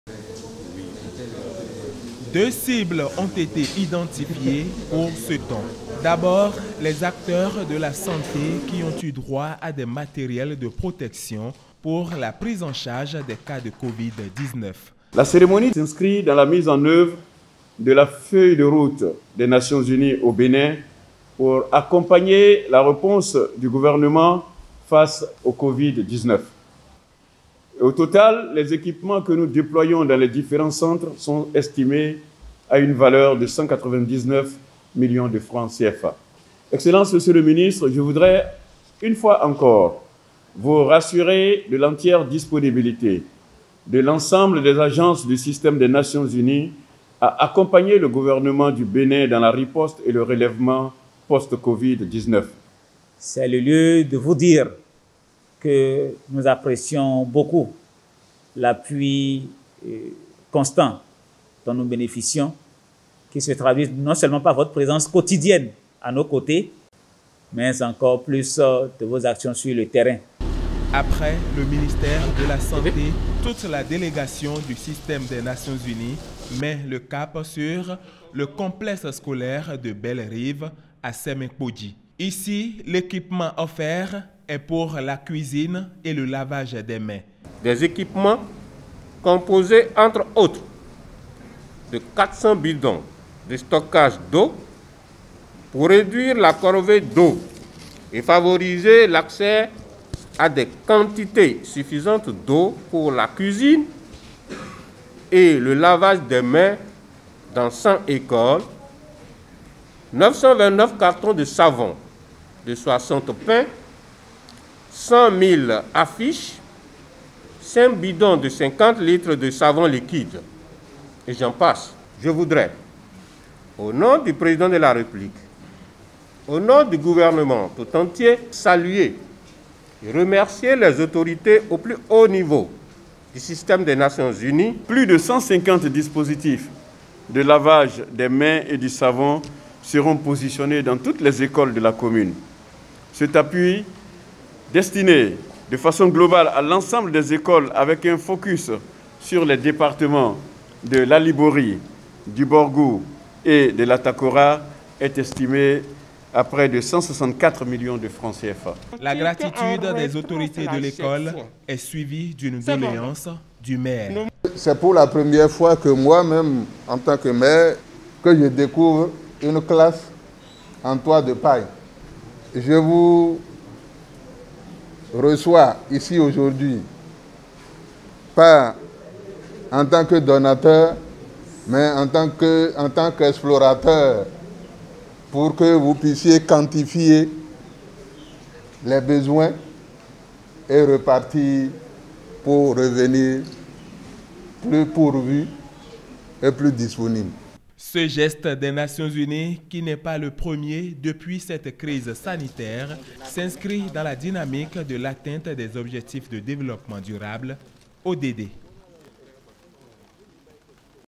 Pandémie du coronavirus : appui des Nations-unies au Bénin
La pandémie du coronavirus continue d’avoir des effets sur les différents secteurs sociaux. Face à la situation, les nations-unies apportent une fois encore leur soutien au Bénin. Le don au profit des acteurs de la santé et des écoles a été transmis ce 18 JUIN au ministère de la santé